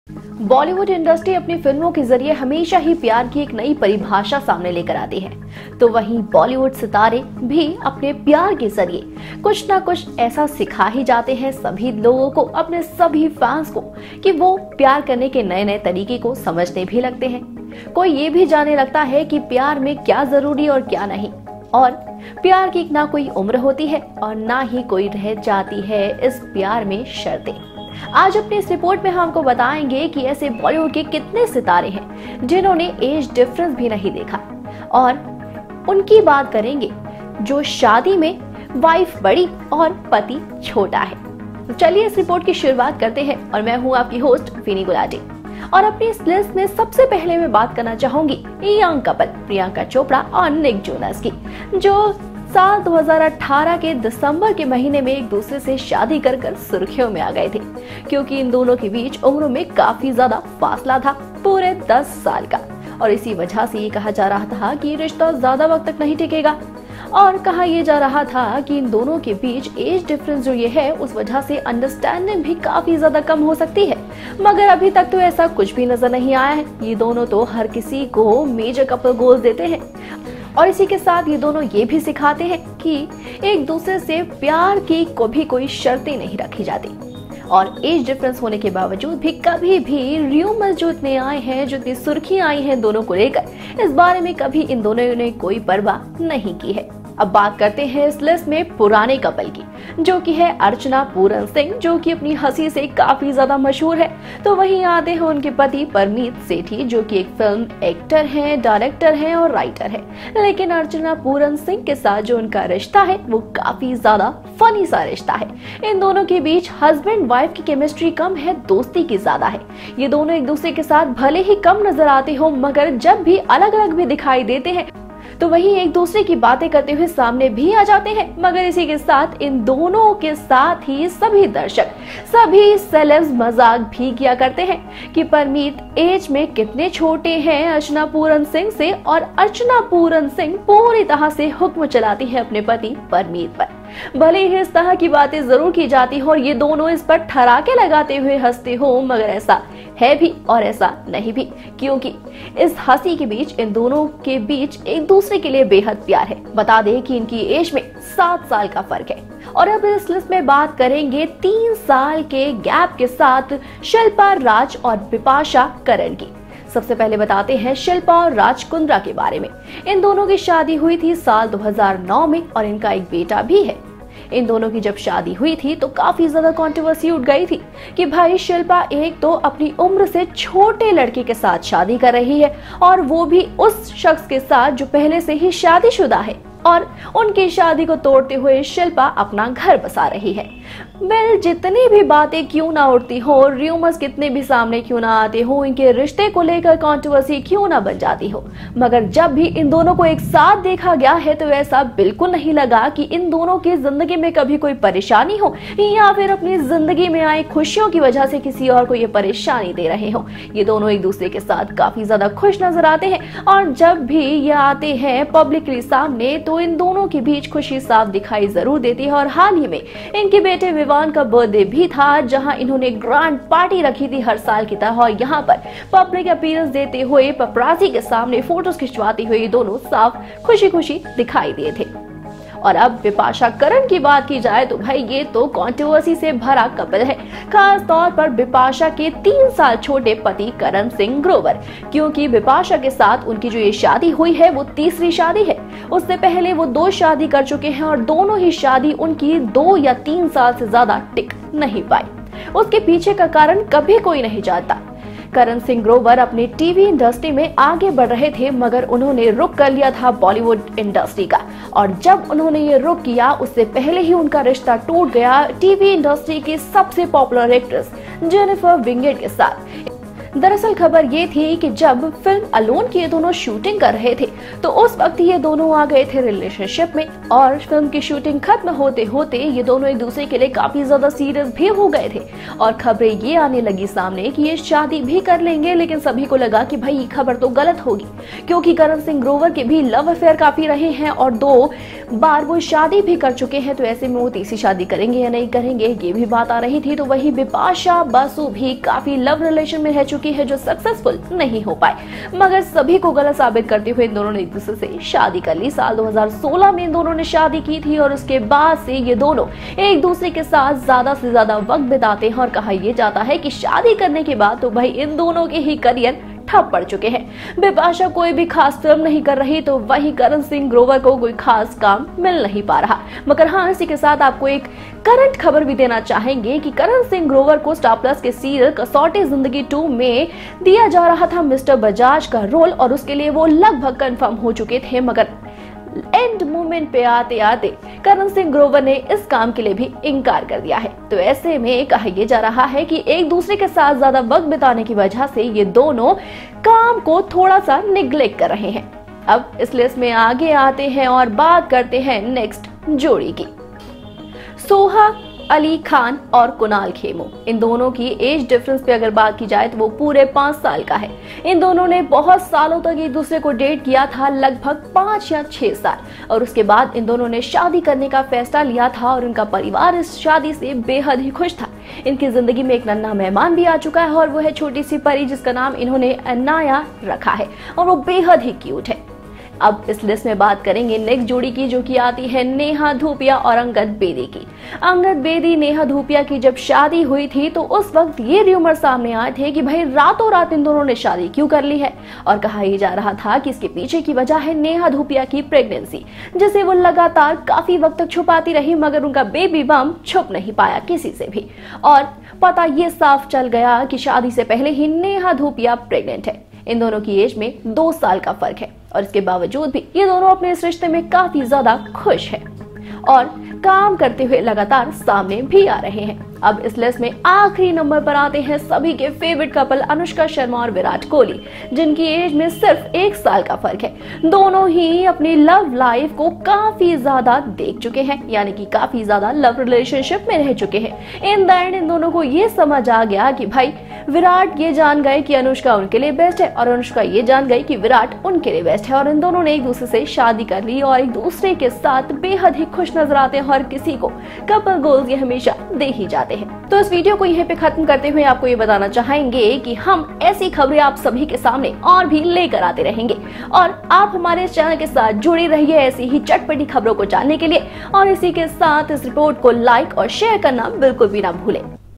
उम्र में छोटा पति बड़ी पत्नी | Shocking AGE Difference of Bollywood Couples
When it comes to love, age, money, height and distance are all just numbers; and they don’t matter. This adage holds true for many of our Bollywood couples who have tied the knot despite the massive age gap they share between them. There was a time when a woman younger than her husband was considered normal, but now times are changing. Today in this video we are telling you about 7 Couples in which husband is younger than wife.